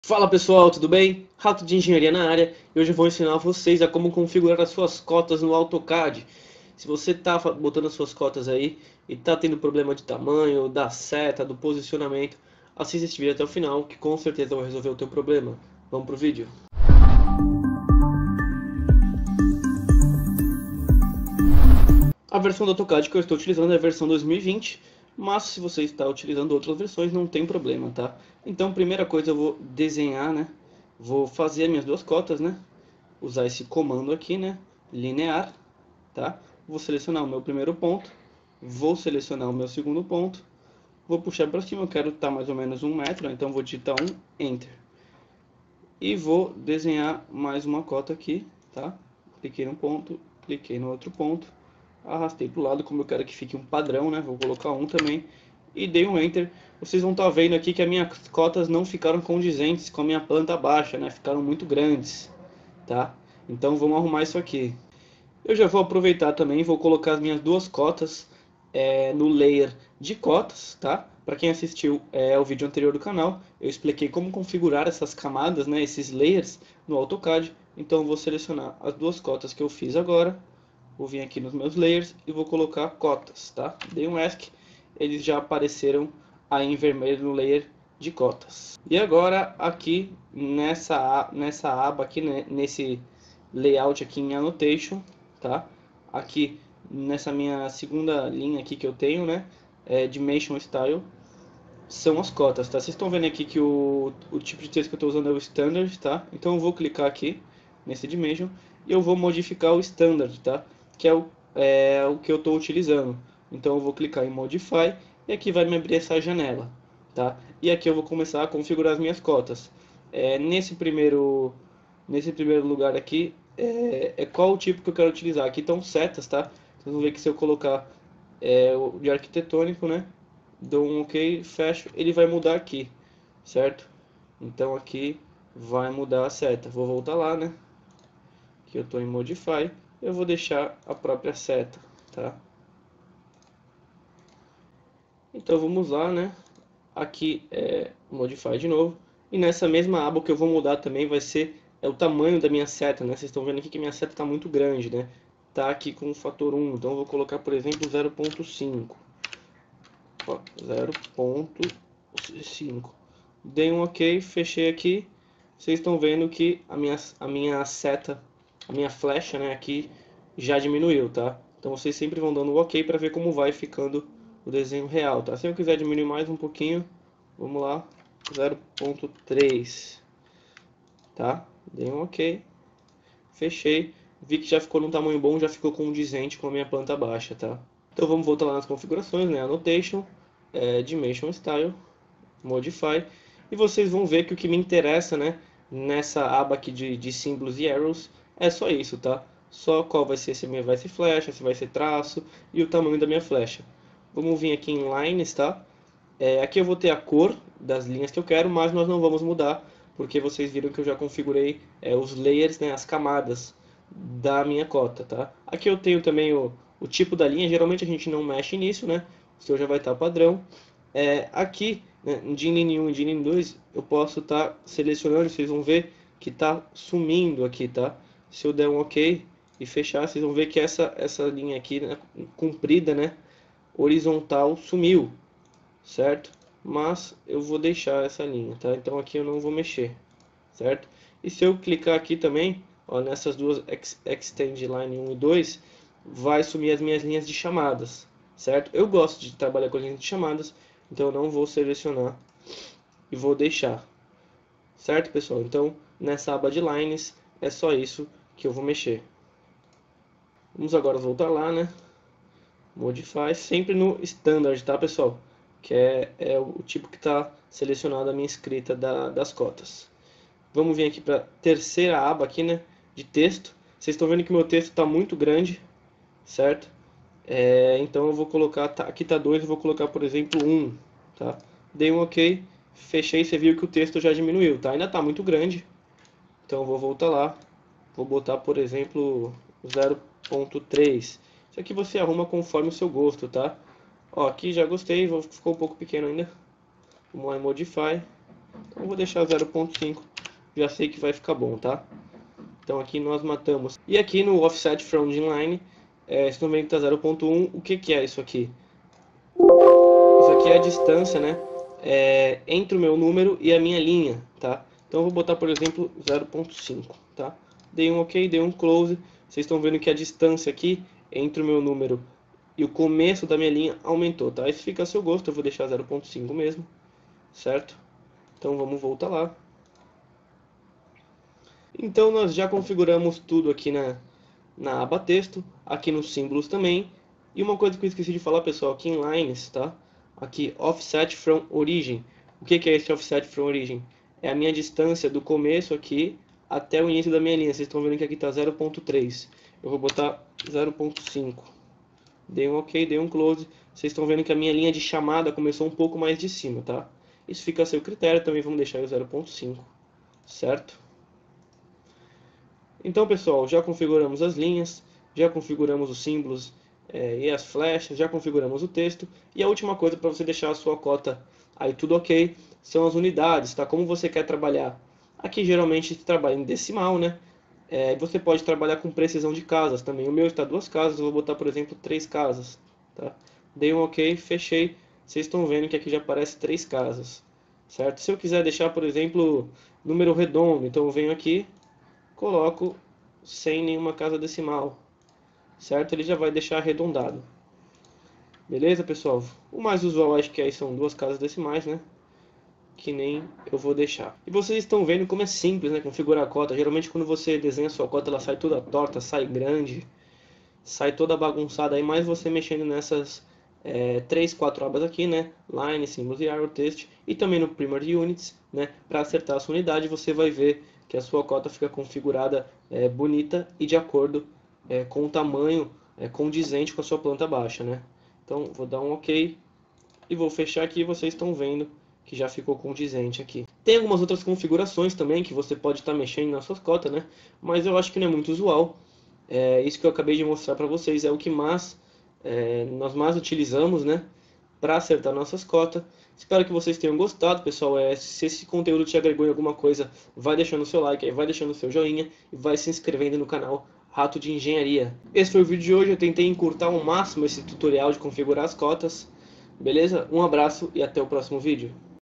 Como criar e configurar COTAS no AutoCAD!
Aprenda rapidamente como você pode criar o seu padrão de Cotas de acordo com o seu desenho e projeto.

As cotas não são nada mais que toda e qualquer medida expressa em desenhos, figuras, planta arquitetônicas e projetos.

São linhas que marcam os pontos que limitam um ambiente ou uma parede, especificando o seu valor, normalmente expresso em Metros, Centímetros ou Milímetros.

Os tipo de cotas básicos são lineares, radiais, angulares, de ordenada e comprimento de arco, utilizando o comando COTA ou DIMLIN você pode criar a sua cota de acordo com o que deseja.

É preciso você Configurar as suas cotas para o tipo de unidade que deseja trabalhar, para elas ficarem compatível com as suas figuras.

Outros Cursos e Produtos:
💯 PORTFÓLIO DE PROJETOS, TEMPLATES E BLOCOS

► Ative as notificações e curta o nosso vídeo